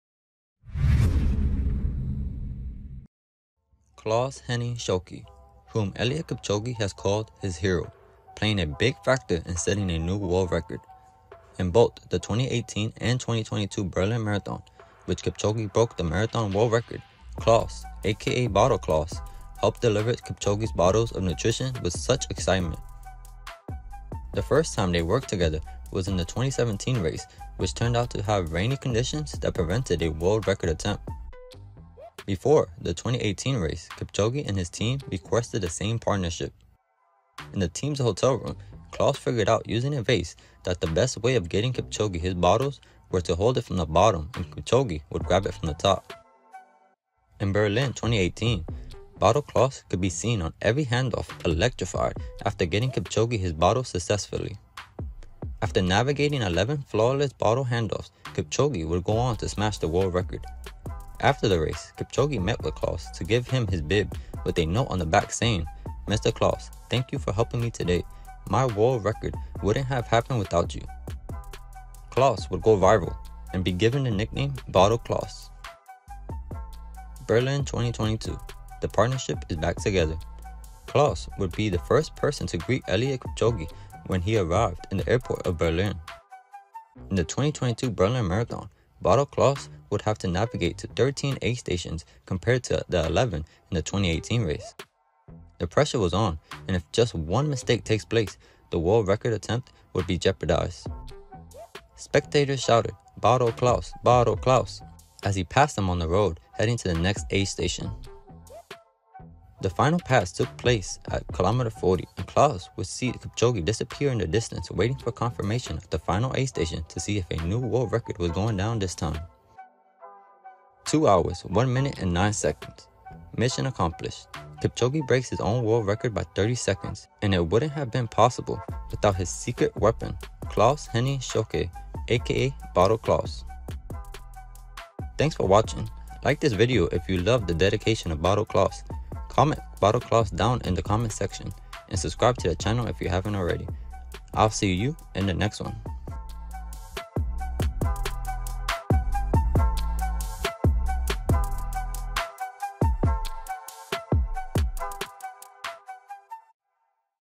Eliud Kipchoge's SECRET WEAPON - Bottle Claus!
Claus-Henning Schulke, who you might know as Bottle Claus, helped Eliud Kipchoge break the marathon world record in both the 2018 and 2022 Berlin Marathon.

Comment BOTTLE CLAUS if you love his excitement.